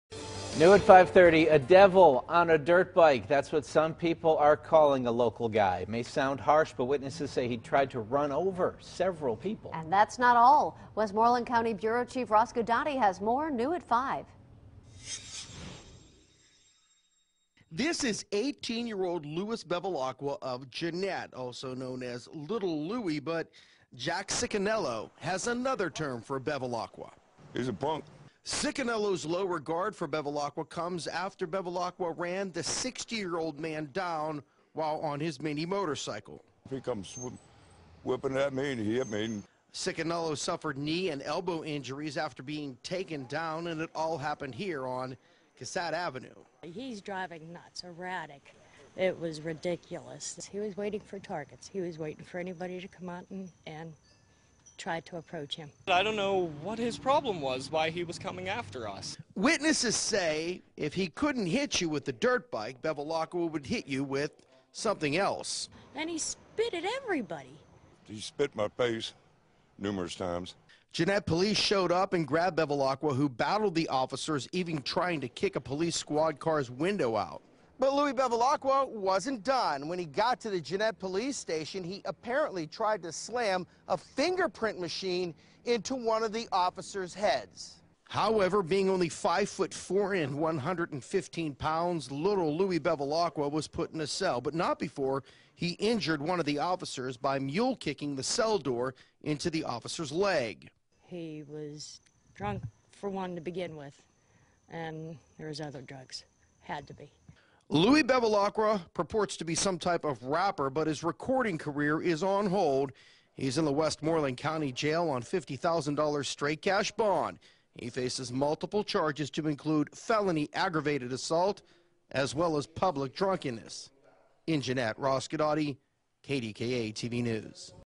Police: Man Arrested For Harassing Neighbors Tried To Hit Officer With Finger Print Machine
He's being called a "devil on a dirt bike" for harassing neighbors, trying to run over several people and assaulting police; KDKA's Ross Guidotti reports. FULL STORY